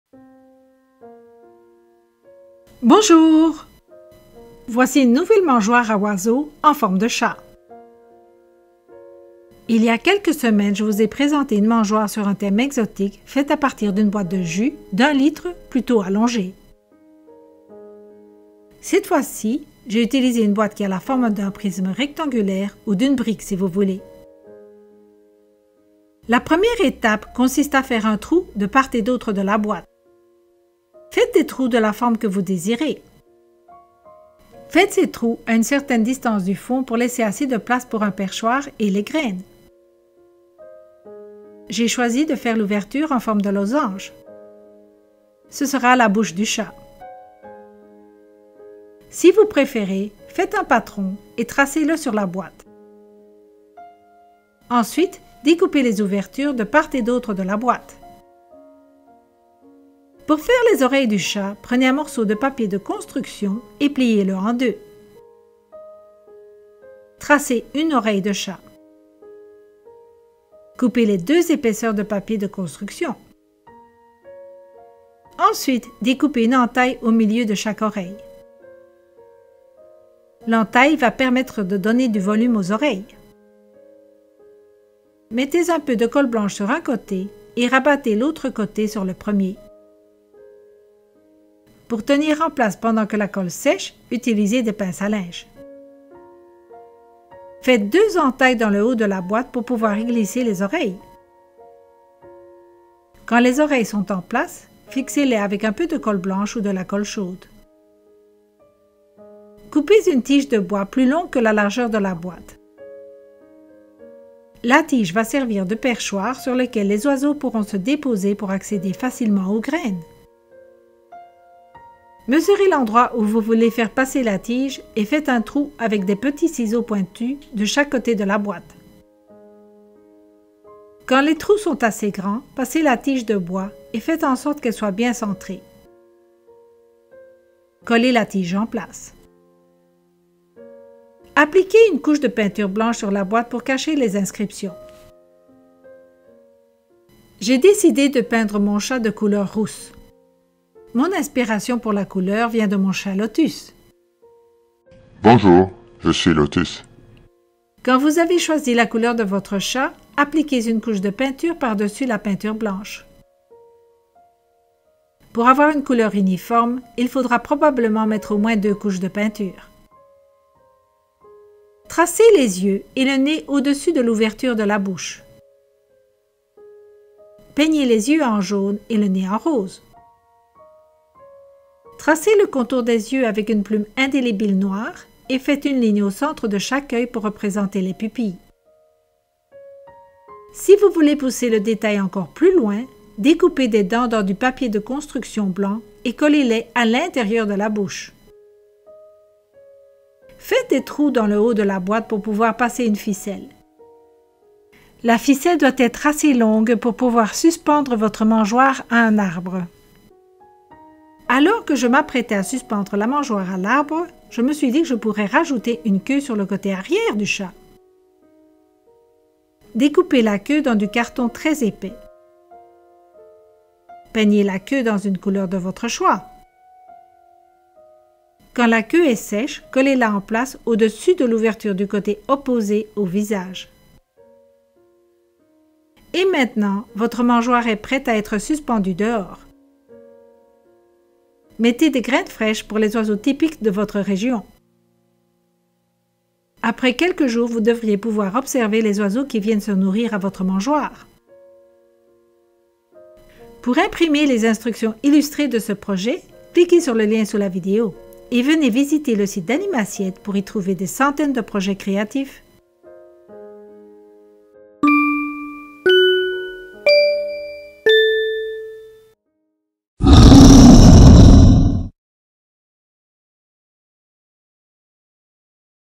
Les oiseaux adorent cette mangeoire en forme de chat
Qui aurait cru que cette mangeoire a plus de succès auprès des oiseaux que mes deux autres mangeoires réunies?  Voyez vous-même, et faites la connaissance de mon chat Lotus.

Image de fond: (c) Can Stock Photo Inc. / colematt, utilisée sous licence